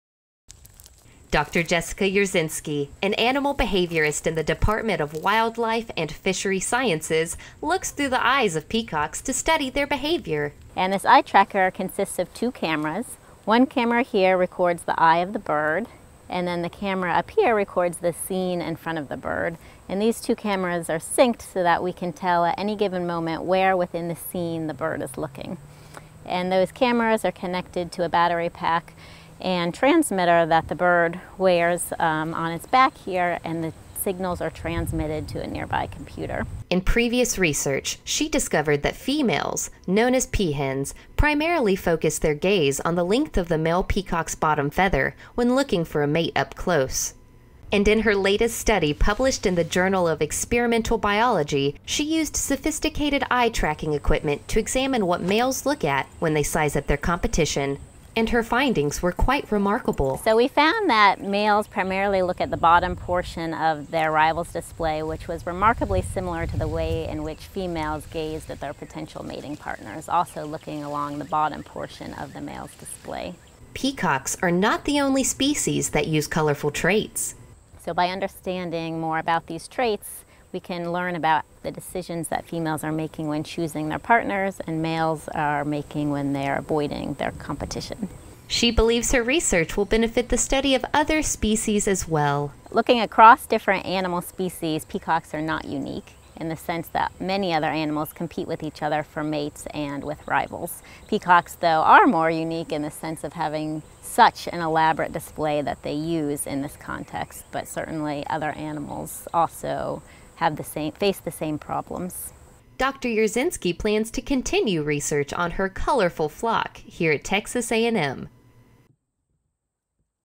Animal behaviorist looks through the eyes of peafowl
Flock, muster ostentation, cluster, lek, pulchritude, pride or bawl, no matter what a group of peacocks is called, Texas A&M University at College Station is now home to 40 of them, thanks to a Texas A&M AgriLife Research scientist.